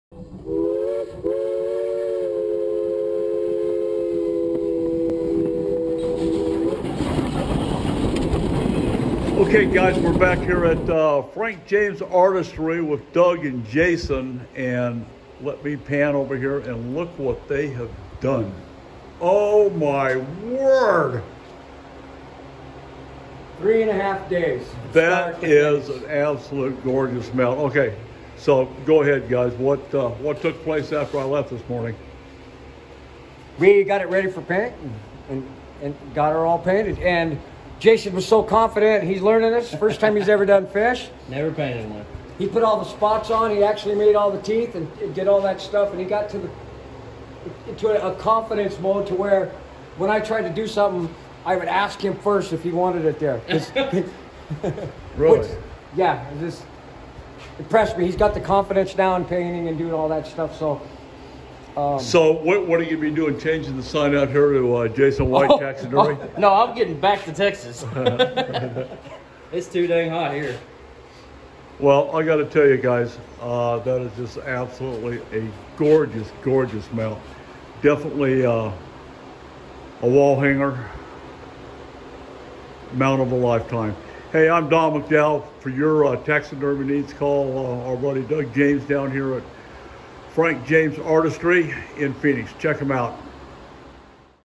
FRANK JAMES TAXIDERMY Alligator Gar
Doug James teaches Jason White on mounting Jason's Alligator Gar